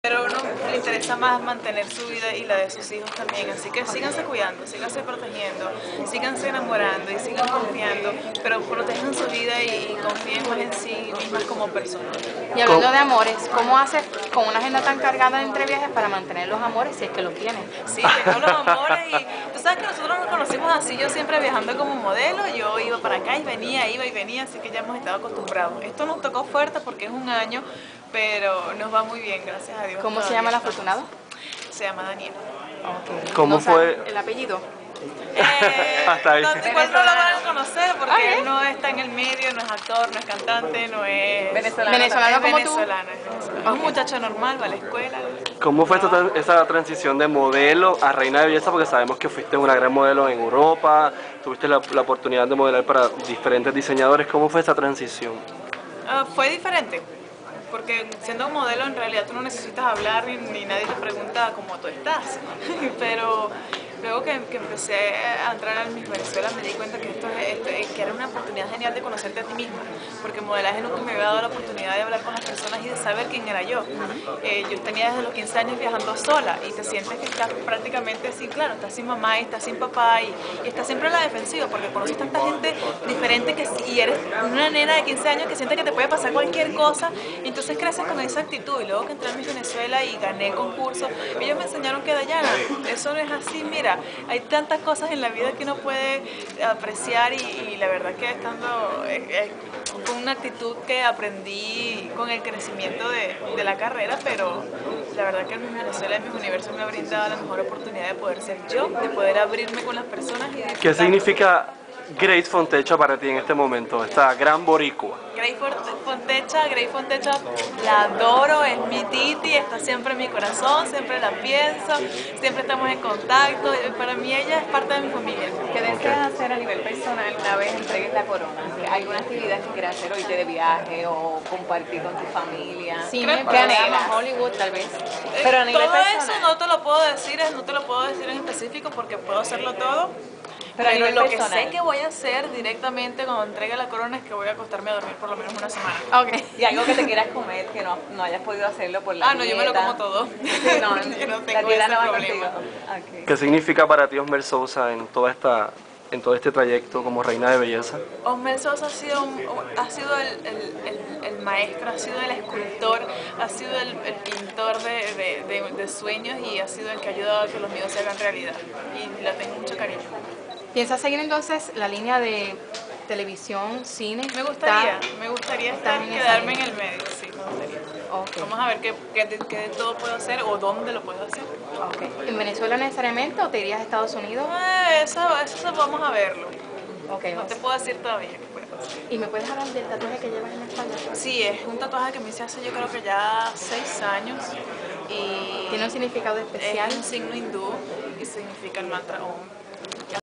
Dayana Mendoza in Puerto Rico - Miss UNIVERSE PR 2010 Press Conference.
Dayana Mendoza in Puerto Rico - Miss UNIVERSE PR 2010 Press Conference. March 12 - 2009 - The Trump International Golf Club Puerto Rico.